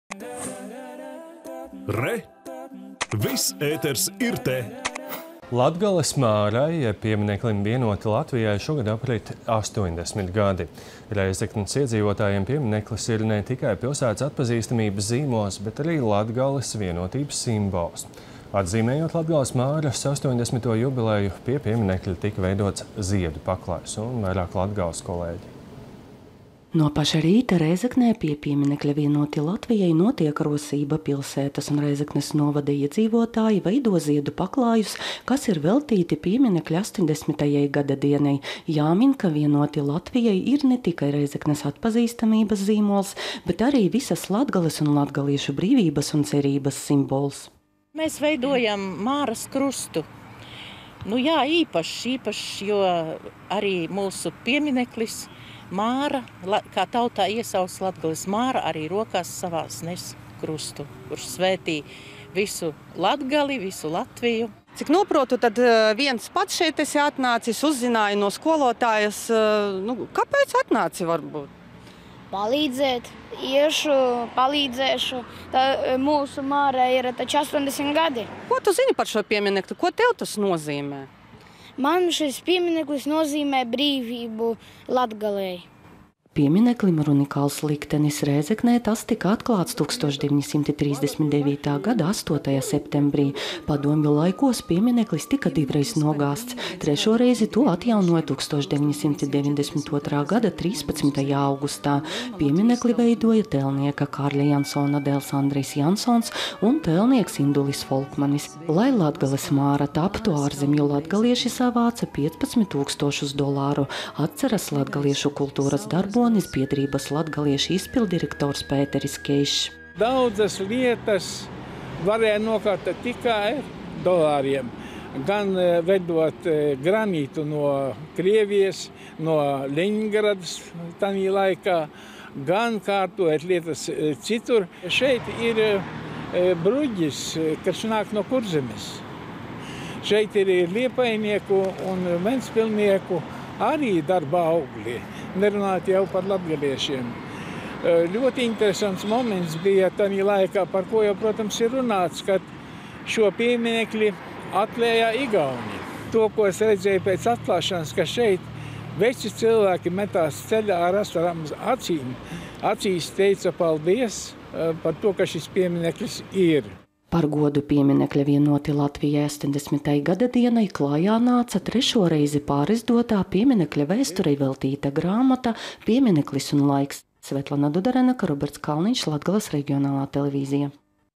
Rēzeknieši svin Latgales Māras 80.gadadienu
Latgales Mārai jeb piemineklim “Vienoti Latvijai” šogad aprit 80 gadi. Rēzeknes iedzīvotājiem piemineklis ir ne tikai pilsētas atpazīstamības zīmols, bet arī Latgales vienotības simbols. Atzīmējot Latgales Māras 80. jubileju pie pieminekļa tika veidots ziedu paklājs. Vairāk Latgales reģionālā televīzija